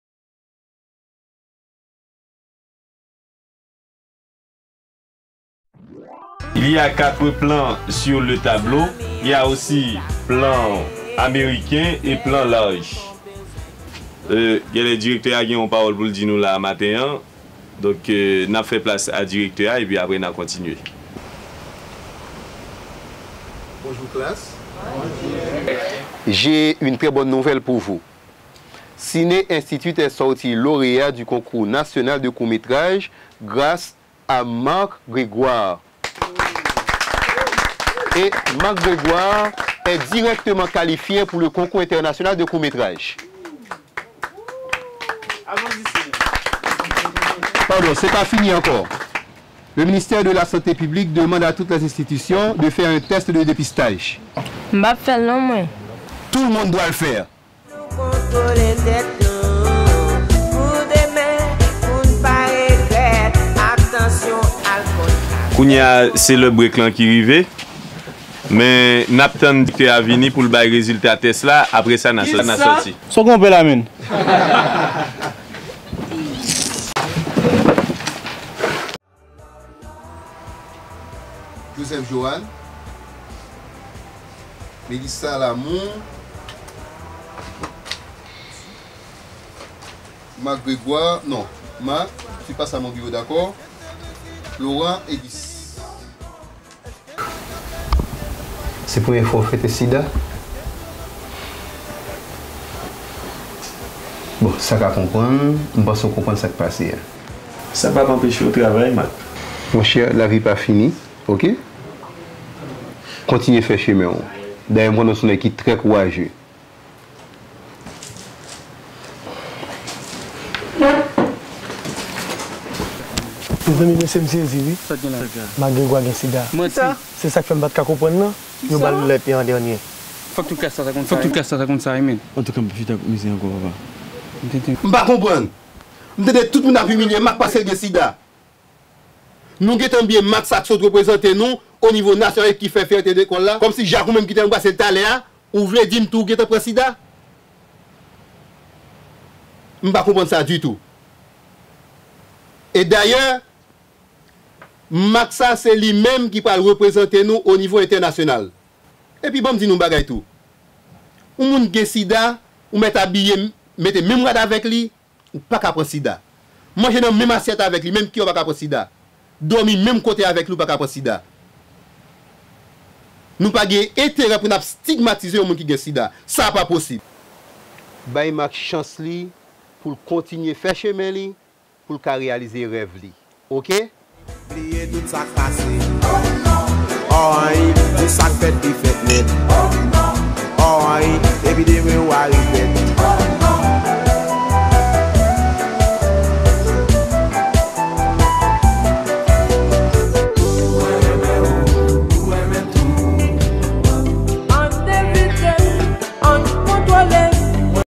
Prejidis / Préjudice / Prejudice
Exercice de 10 Gdes Film à Dialogue de première année d'étude ( Promotion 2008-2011 )

Réalisateur : Yrvelt Lamour
Productrice : Keziah Jean